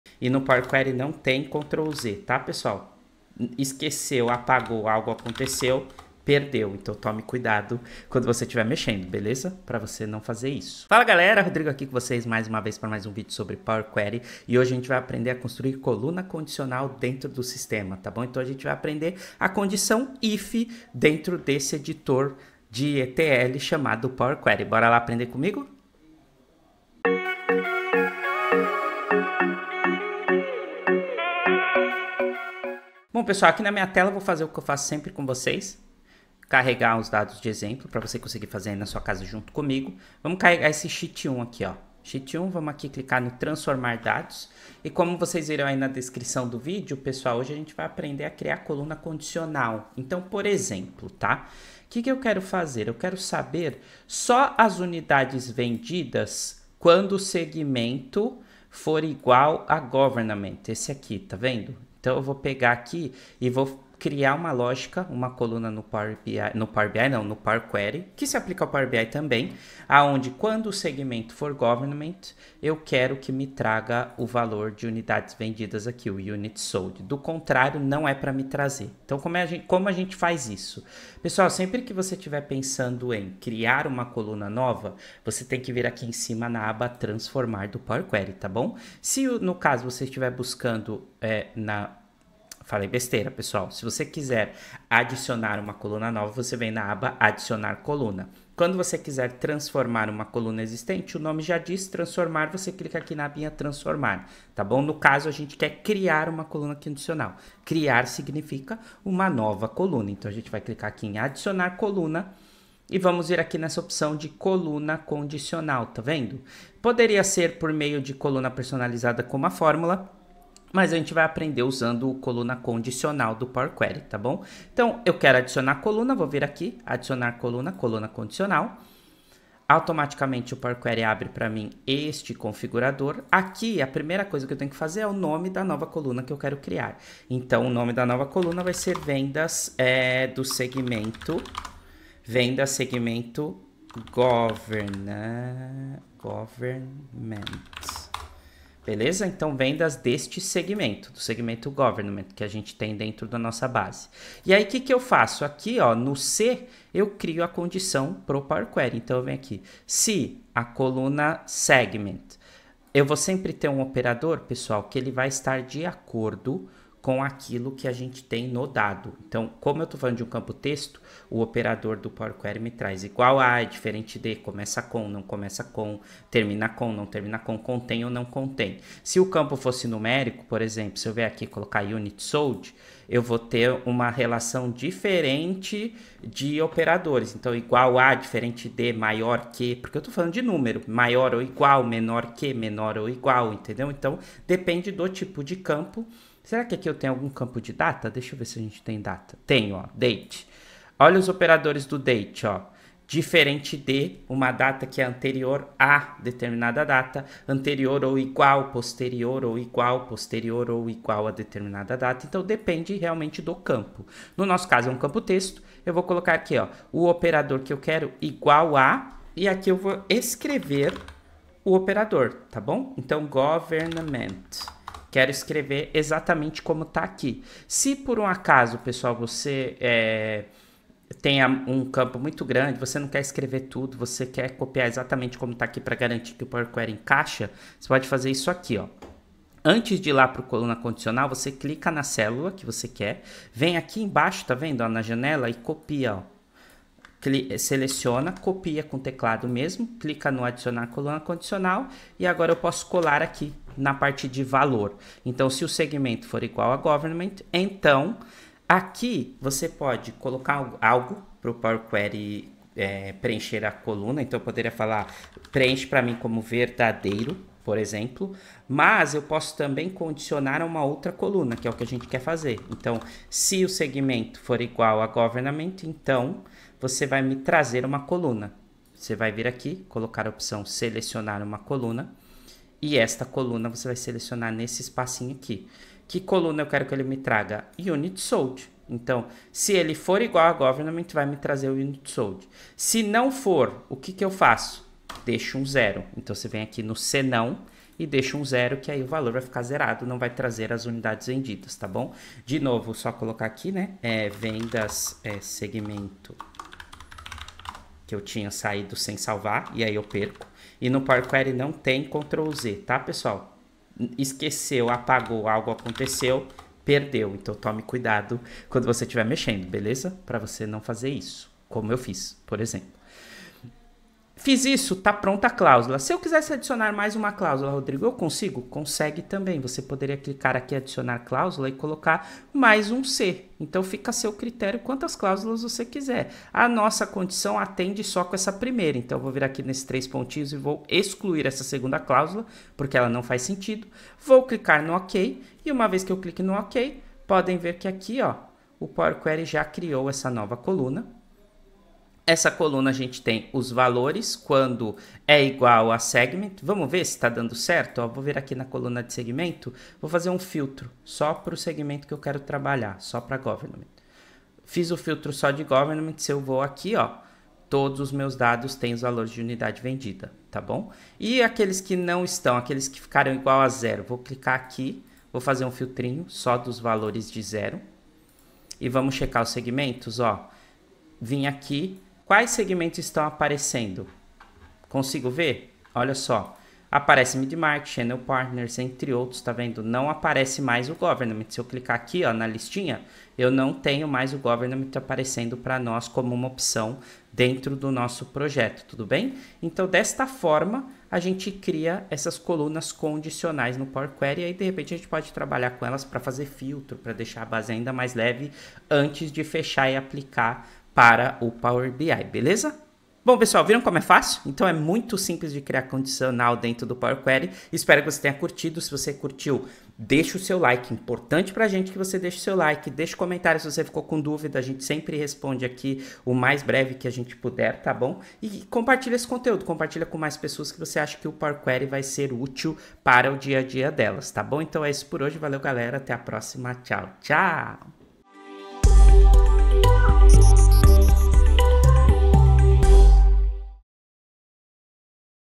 Como fazer uma coluna condicional no Power Query
Desvende o segredo das Colunas Condicionais no Power Query! No vídeo de hoje, o Rodrigo vai guiá-lo pelo processo de criação de colunas condicionais de maneira fácil e eficaz!

🔷 Interesse em conhecer mais? ACESSE AGORA!

00:00:00 Preview
00:00:10 Introdução
00:00:32 Caregando dados de exemplo
00:00:50 Criando Coluna Condicional
00:06:52 Encerramento